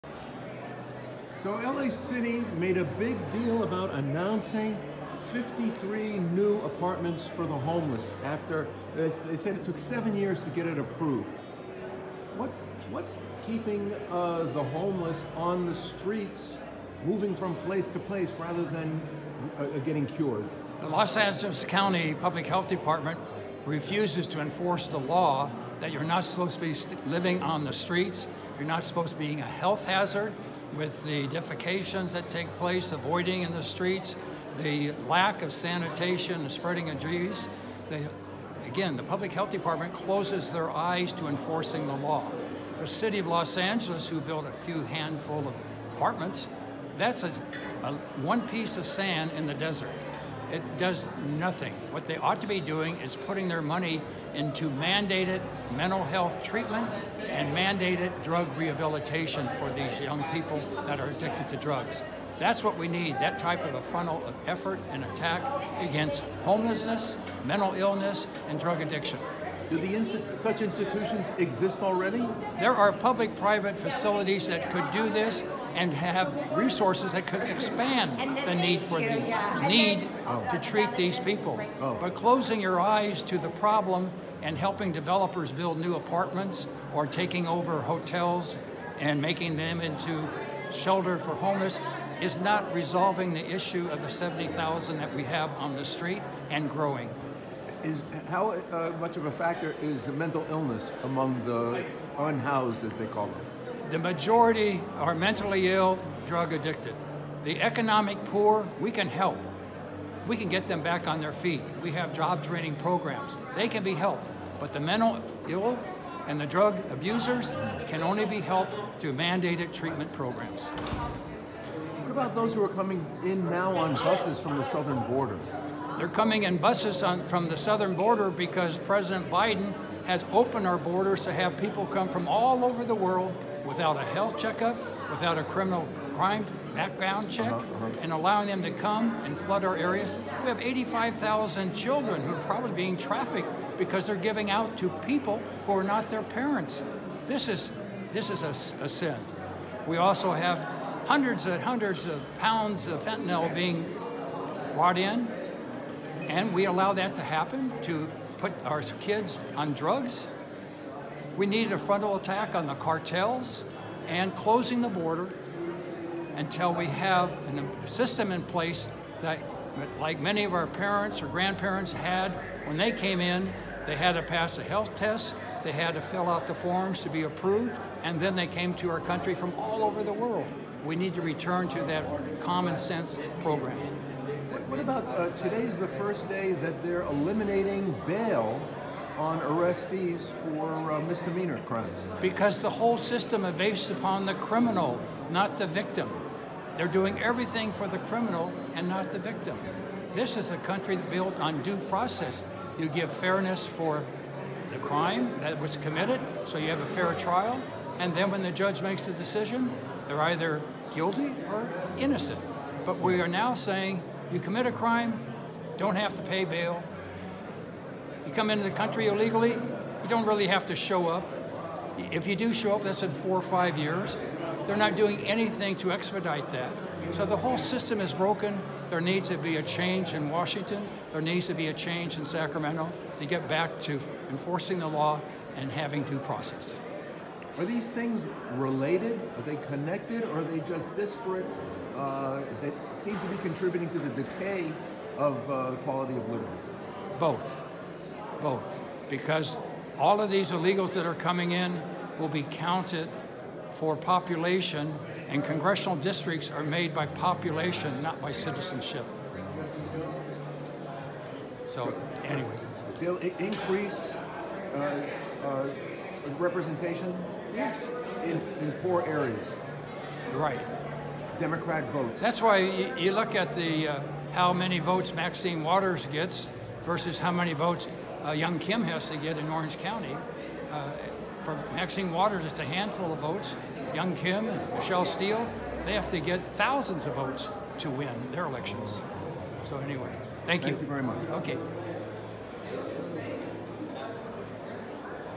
CA Newsom's homeless plan halves fmr Mayor Antonovich's but adds 1000's more illegal aliens to rolls
Interview with former L.A. County Mayor Michael Antonovich at KRLA Town Hall event. Mayor Antonovich highlights the ongoing issue of homelessness in Los Angeles and proposes solutions that prioritize mandatory mental health treatment and drug rehabilitation. He criticizes the lack of enforcement of laws against living on the streets and raise concerns about sanitation issues. Additionally, he expresses worry about the influx of illegal immigrants, asserting that it negatively affects living conditions and representation in poor areas. Antonovich emphasizes the need for changes in Washington and Sacramento to enforce laws and prioritize addressing these issues.

KABC-TV News reported that an alternative mental health court program designed to fast-track people with untreated schizophrenia and other psychotic disorders into housing and medical care - potentially without their consent - kicked off in seven California counties, including Orange and Riverside, on Monday 2 Oct '23.
Calif Gov. Gavin Newsom created the new civil court process, called "CARE Court," as part of a massive push to address the homelessness crisis in California. Lawmakers approved it despite deep misgivings over insufficient housing and services, saying they needed to try something new to help those suffering in public from apparent psychotic breaks.  Residents dismayed by the estimated 171,000 homeless people in California cheered at the possibility of getting them help and off the streets. Critics blasted the new program as ineffective and punitive given that it could coerce people into treatment. It's not clear who the program might help nor how effective it will be. That's because the eligibility criteria is narrow and limited largely to people with untreated schizophrenia and related disorders. Severe DEPRESSION, BIPOLAR DISORDER, AND ADDICTION (BY ITSELF) DO NOT QUALIFY!